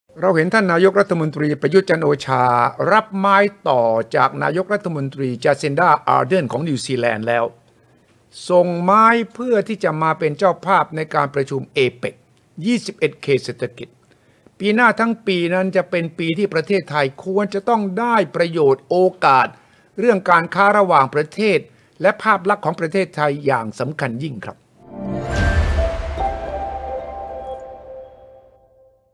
Suthichai 5 minutes จากนายกฯกีวีสู่นายกฯตู่
สุทธิชัย เล่า นายกกีวี ได้ส่งไม้ต่อให้นายกตู่แล้ว การประชุม APEC ครั้งนี้ ไทยต้องได้ประโยชน์และโอกาสเรื่องการค้าระหว่างประเทศ!